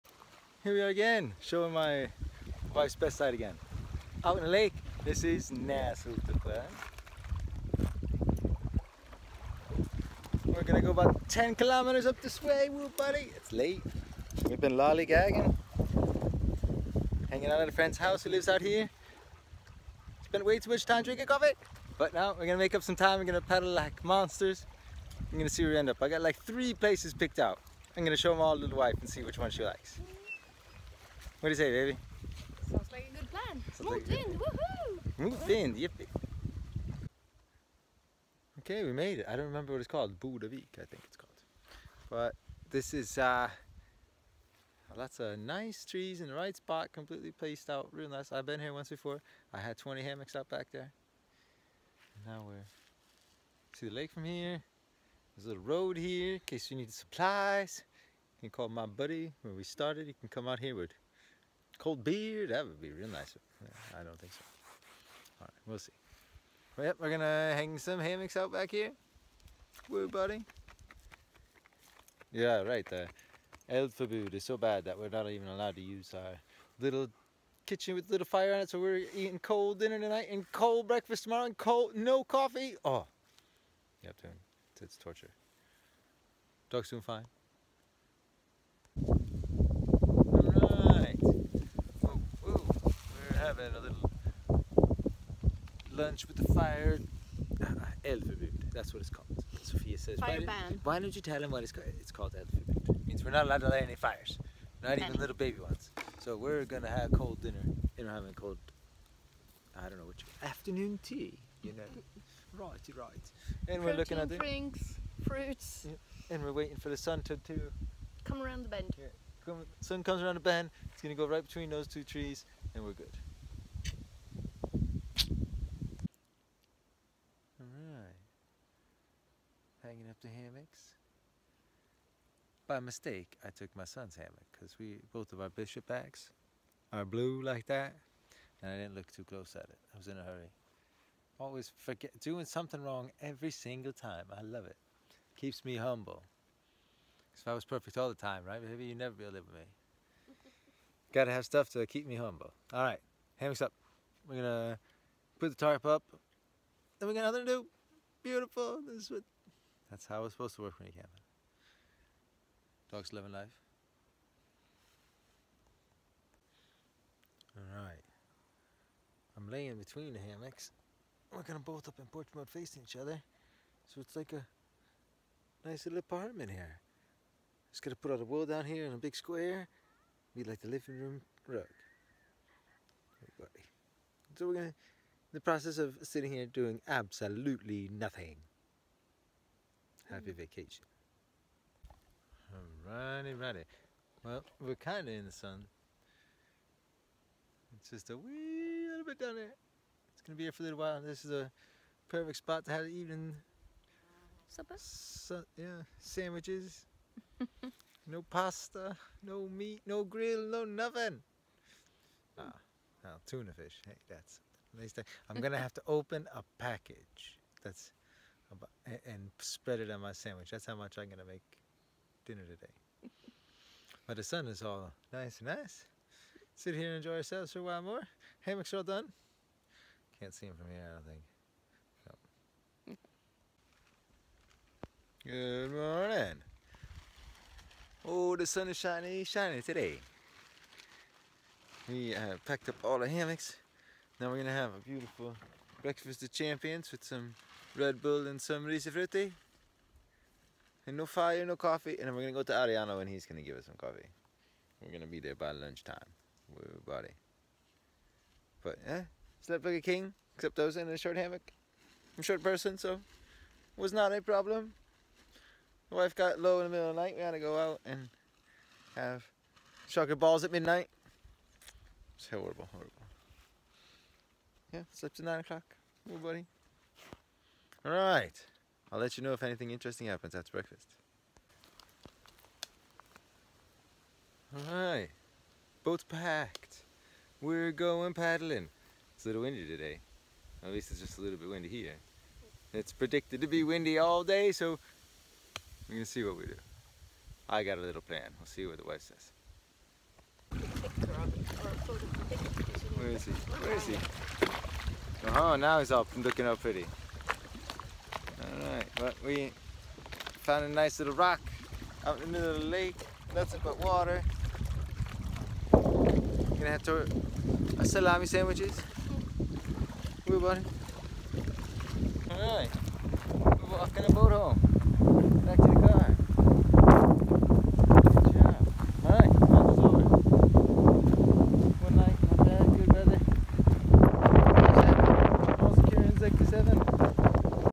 Paddling Näshultasjön Summer 2018
Revisiting an old campsite in Näshulta.